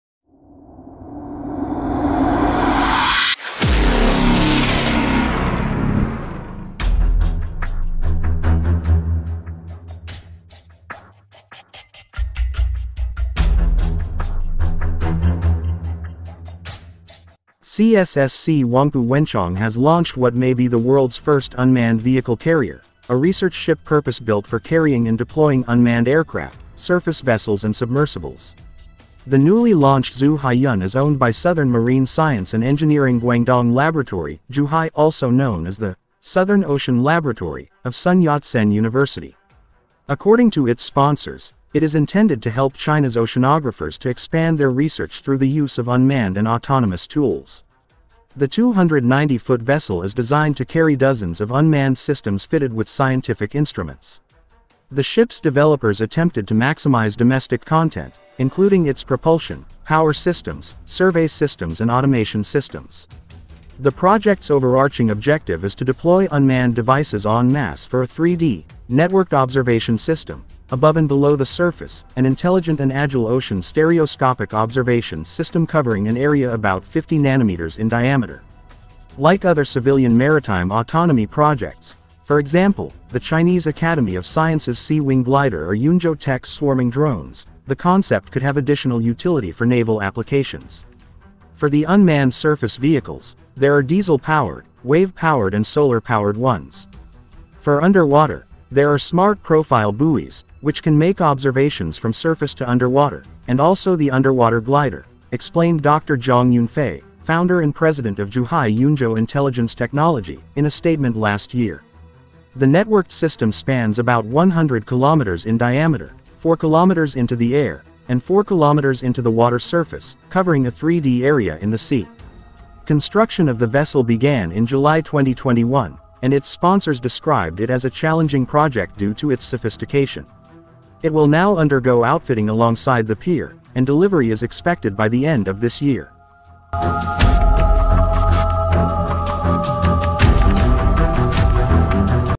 First In The World's! China Launches Zhu Hai Yun World's First Drone Carrier
CSSC Huangpu Wenchong has launched what may be the world's first "unmanned vehicle carrier," a research ship purpose-built for carrying and deploying unmanned aircraft, surface vessels and submersibles.